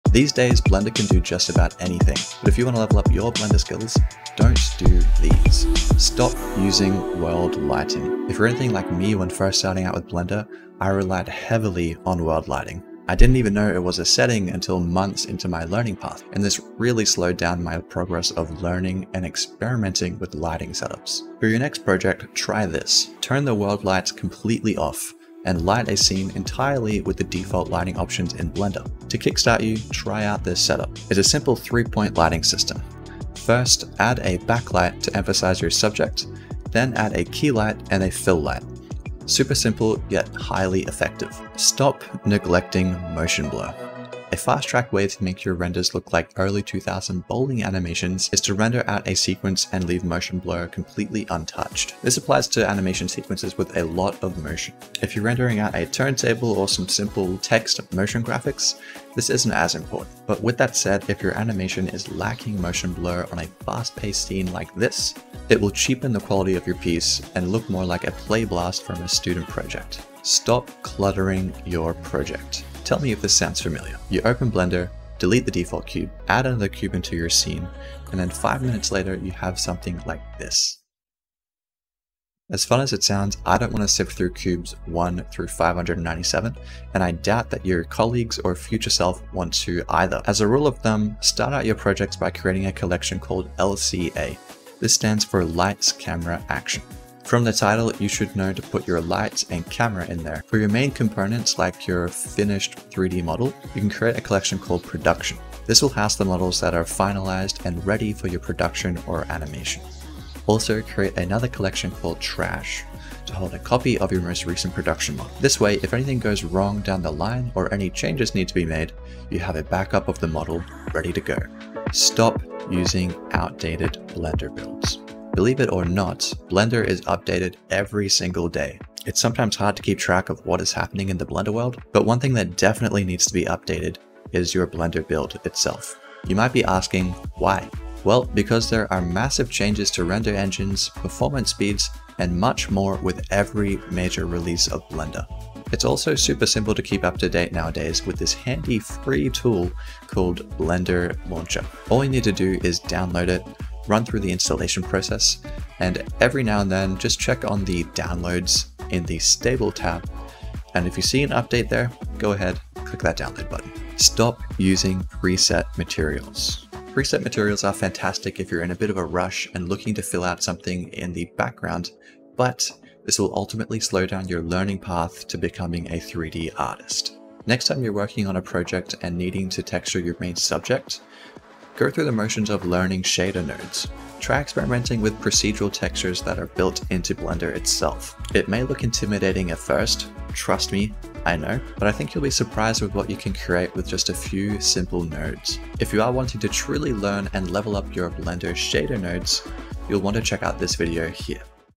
5 Things You Should STOP Doing In Blender
Blender can be hard to conquer, we all make mistakes and we learn from them. I've summed up the 5 most common mistakes, watch this video in full to learn how you can avoid them and become a Blender Generalist in no time!

         🎨Try SkillShare FREE For a Month! 🎨

📙Helpful Blender Addons! 📙

🎥My Essential Gear Setup 👇
▶️ Sony a6400
▶️ A Cam Lens
▶️ B Cam Lens
▶️ Key Light

Chapters 📚
0:00 Introduction
0:07 STOP USING WORLD LIGHTING
0:42 STOP NEGLECTING MOTION BLUR
1:12 CLUTTERING YOUR PROJECT
2:02 STOP USING OUTDATED
2:45 STOP USING PRE-SET MATERIALS